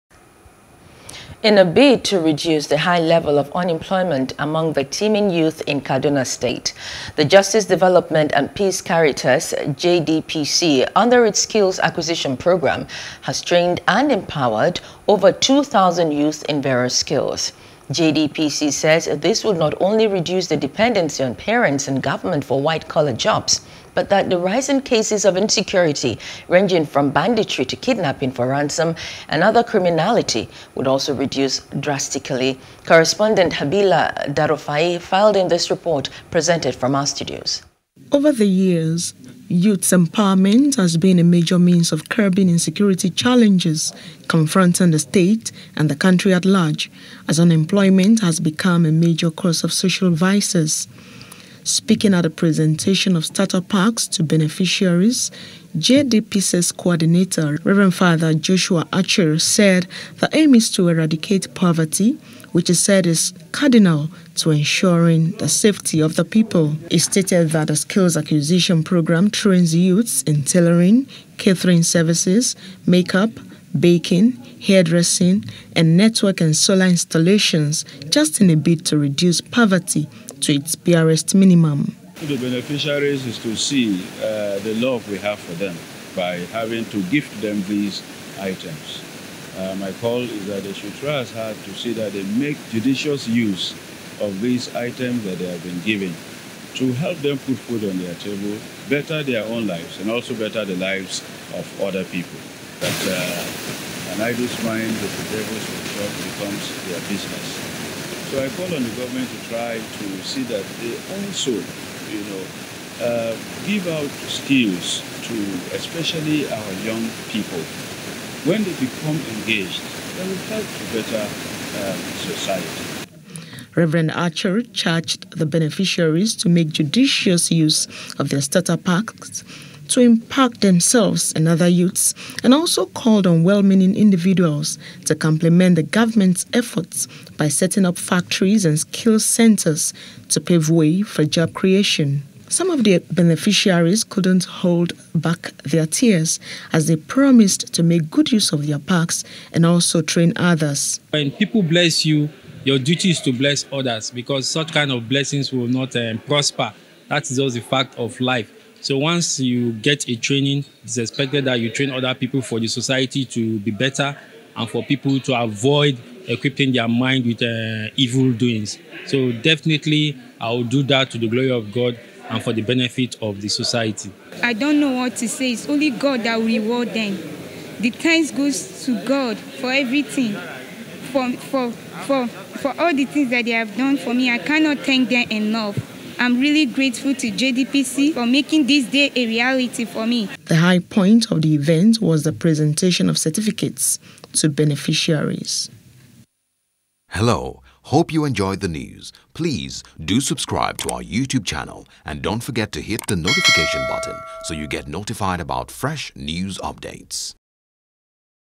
J.D.P.C: Over 2000 Youths Empowered In Various Skills | NEWS
In a bid to reduce the high level of unemployment among the teeming youths in Kaduna State, the Justice Development and Peace Caritas (J.D.P.C.) under its Skills Acquisition Program, has trained and empowered over two thousand youths in various skills.